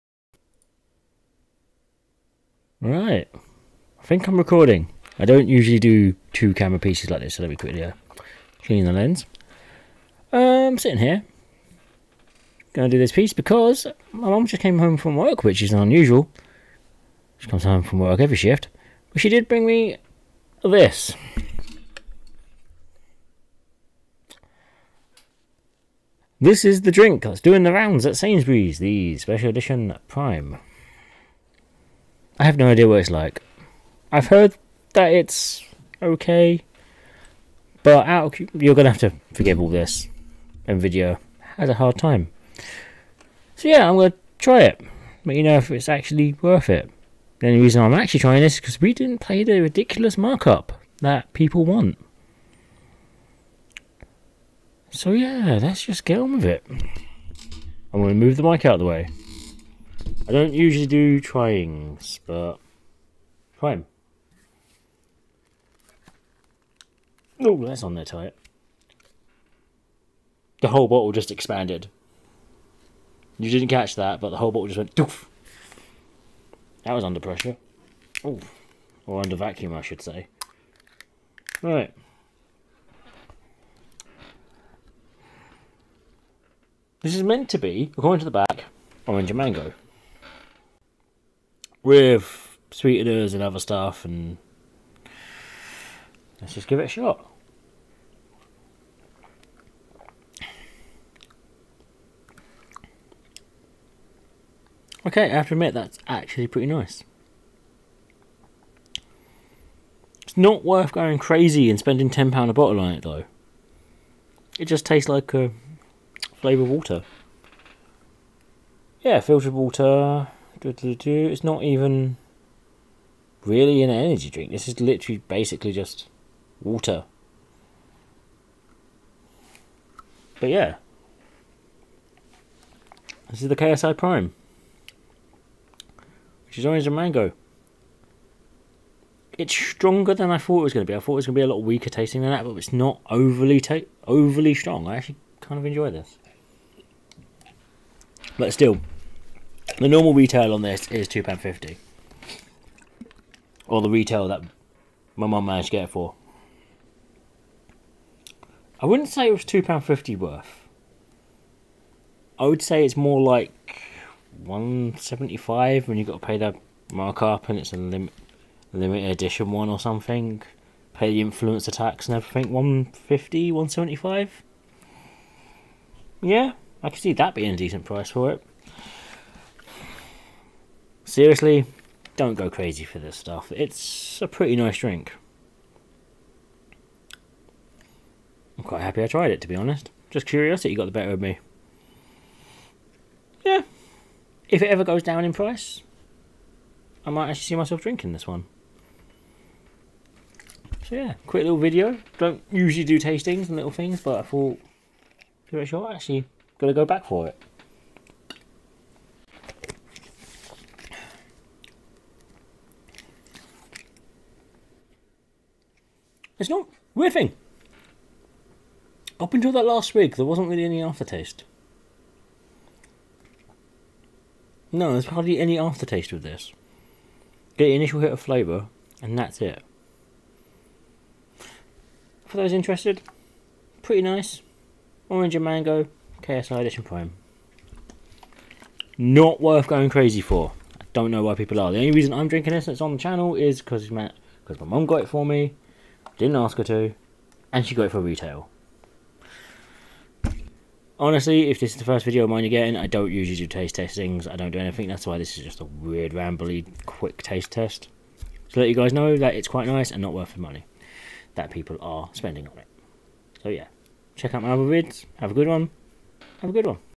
KSI Prime... Worth the price?
quick video on the KSI edition Prine drink, Didn't say in the video but it tastes kinda similar as a drink in the UK called Rio, Not sure if over countries have Rio.